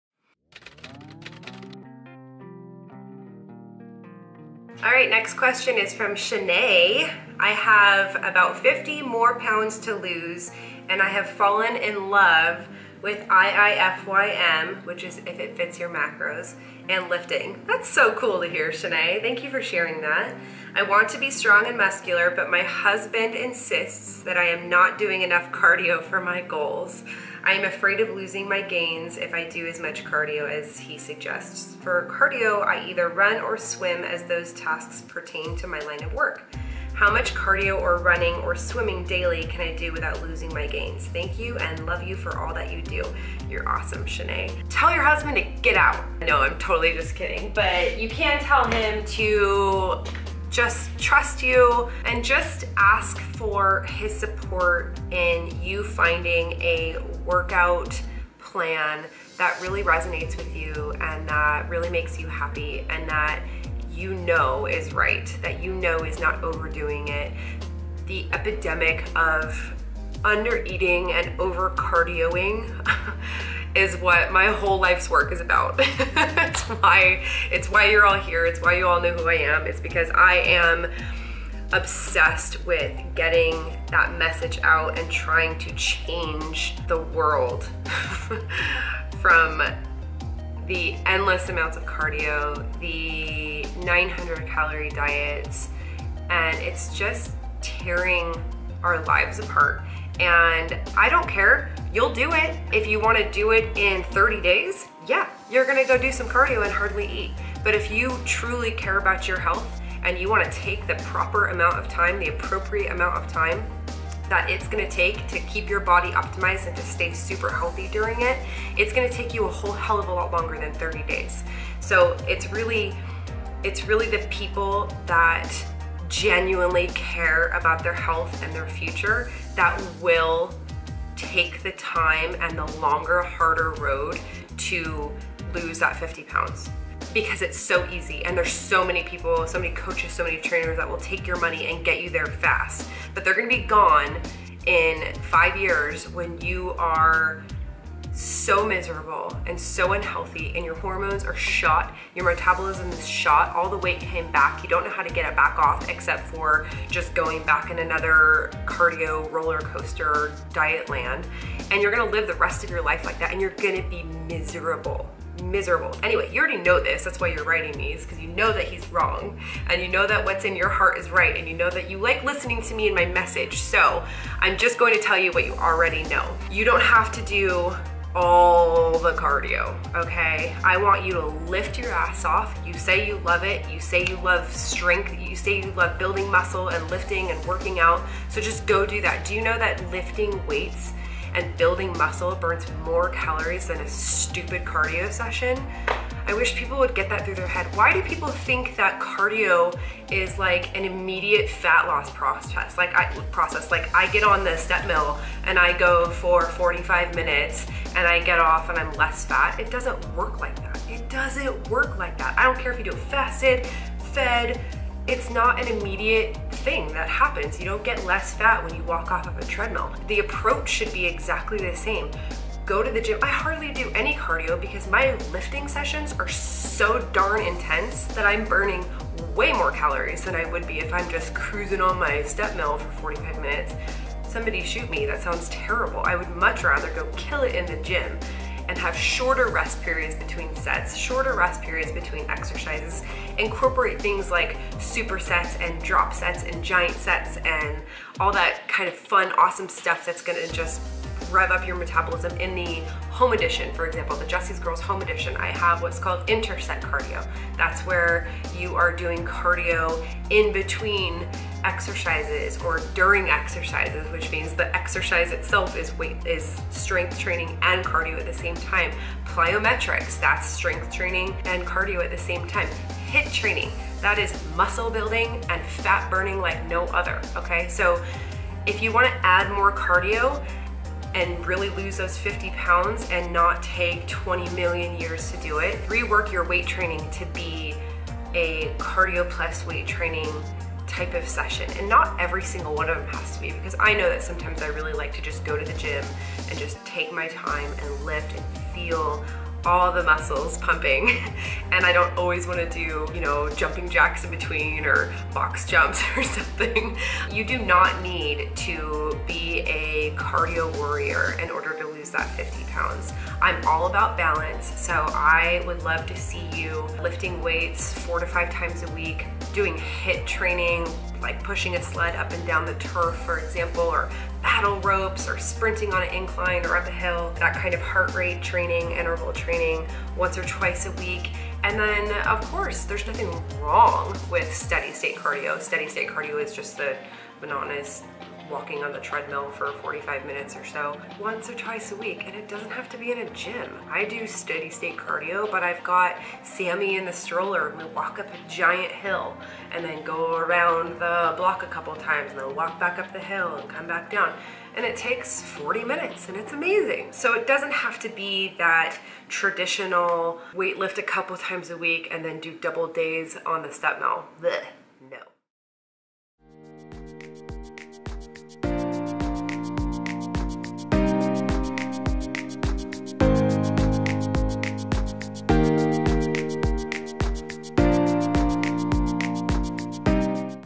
Cardio or Cardi-no?
Ahh the big C word!! We are talkin' about cardio, people! Should we do it or should we not? Is is better than just lifting weights all day? Watch and find out! ;)